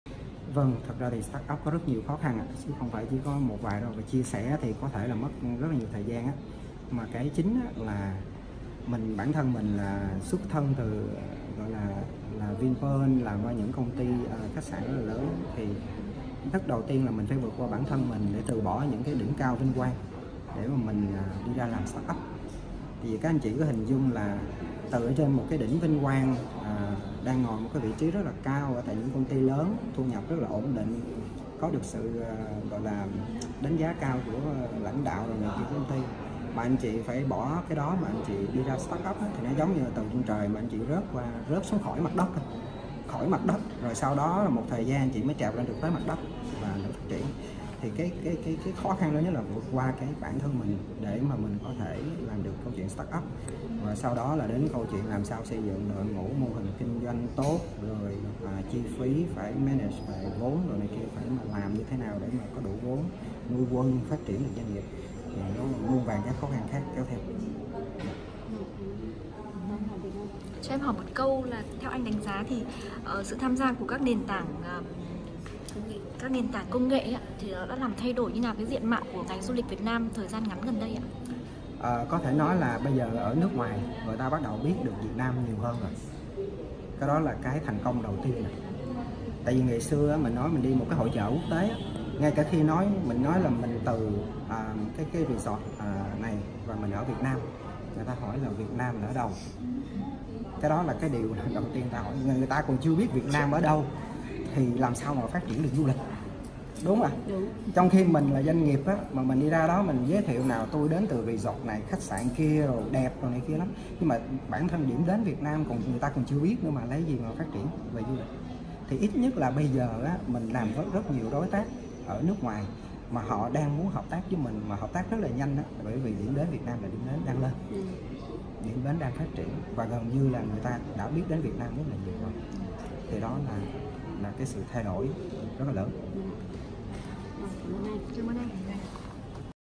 Những ngày đầu gian khó của người sáng lập nền tảng Bedlinker
Bedlinker.com là sàn giao dịch thương mại điện tử B2B (doanh nghiệp với doanh nghiệp) đầu tiên dành cho lĩnh vực du lịch và khách sạn tại Việt Nam, ra đời năm 2017. Sàn giao dịch mở này là nơi các công ty du lịch được kết nối trực tiếp tới lượng lớn các khách sạn, khu nghỉ dưỡng và nhiều loại hình lưu trú khác; nơi các công ty du lịch có thể dễ dàng kiểm tra và đặt phòng ngay lập tức với khách sạn ưa thích của mình.

Và dự án Bedlinker và giành giải Nhất chung kết cuộc thi "Doanh nghiệp khởi nghiệp sáng tạo ứng dụng công nghệ thông tin phát triển du lịch" diễn ra sáng nay (18/6), tại Hà Nội.

Đào Quang Thuận, người sáng lập dự án,  đã có những chia sẻ về những ngày đầu khó khăn khi vận hành nên tảng công nghệ này.